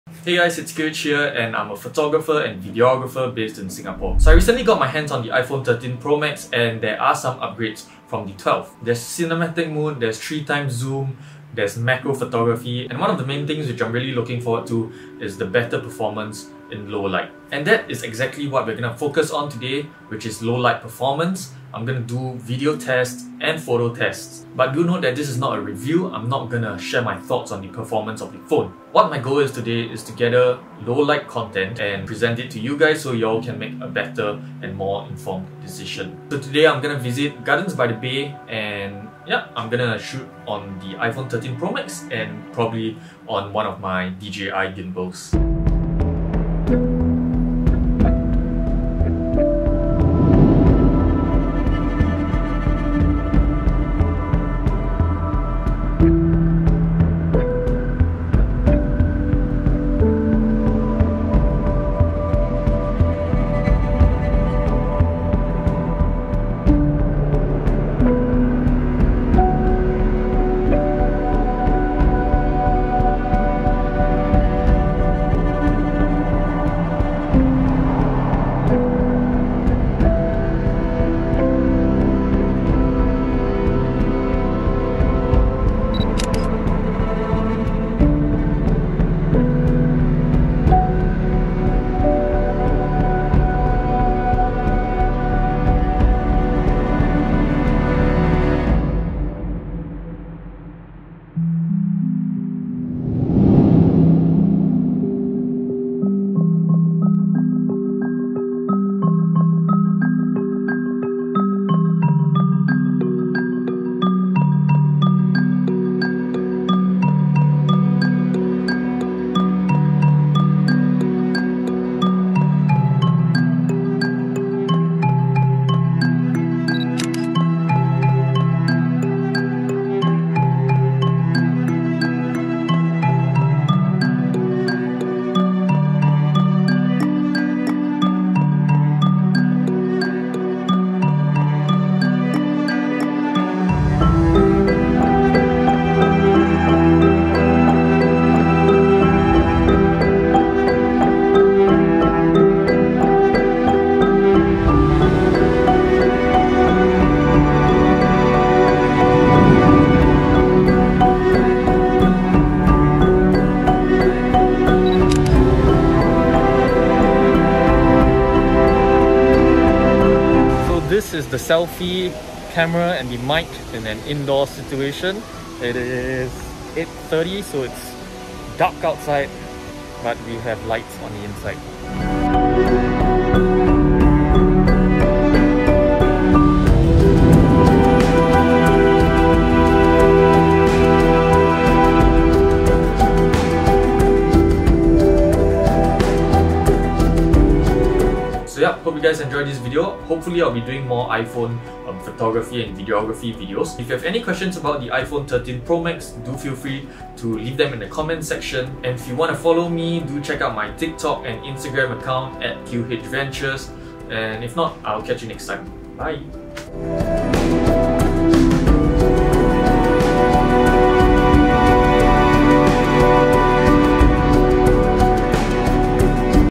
iPhone 13 Pro Max Low Light Photo and Video Test | Gardens by the Bay Singapore
iPhone 13 Pro Max Low Light Photo and Video Test! Let me know what you guys want to see next!

0:00 Intro
0:45 1x Zoom Lens
1:37 0.5x Zoom Lens
2:37 3x Zoom Lens
2:50 Front Camera
3:02 Cinematic Mode
3:18 Outro